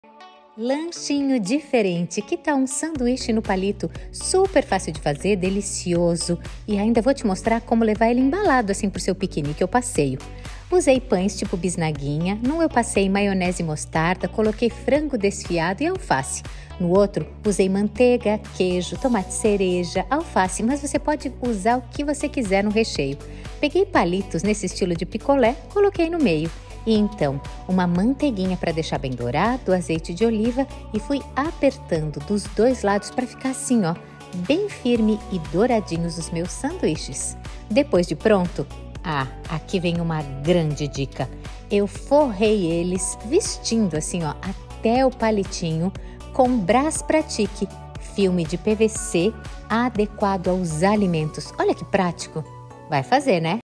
Sanduiche no Palito?
SANDUICHE NO PALITO? Sim! Um lanchinho diferente, prático pra levar no piquenique, nos passeios, na mochila escolar…

Você pode usar outro tipo de pão, variar nos recheios, conforme seu gosto pessoal. Eu preparei na frigideira, mas poderia ser também numa sanduicheira/torradeira.

Coloca a imaginação pra funcionar, e cria sua versão. Depois me conta!

Curtiu o video? Me segue e compartilha!